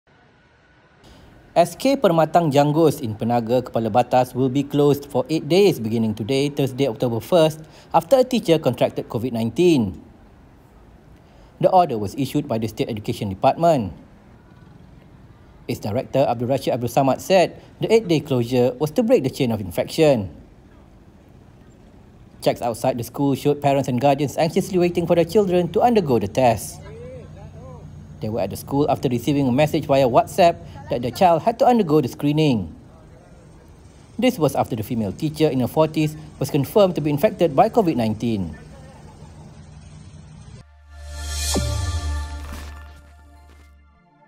Teacher down with Covid 19, SK Permatang Janggus to close for 8 days
SK Permatang Janggus in Penaga, Kepala Batas will be closed for eight days, beginning today, Thursday, October 1st, after a teacher contracted Covid-19.

The order was issued by the state Education Department.

Its director Abdul Rashid Abdul Samad said the eight-day closure was to break the chain of the infection.

Checks outside the school showed parents and guardians anxiously waiting for their children to undergo the test.

They were at the school after receiving a message via WhatsApp, that their child had to undergo the screening.

This was after the female teacher, in her 40s, was confirmed to be infected by Covid-19.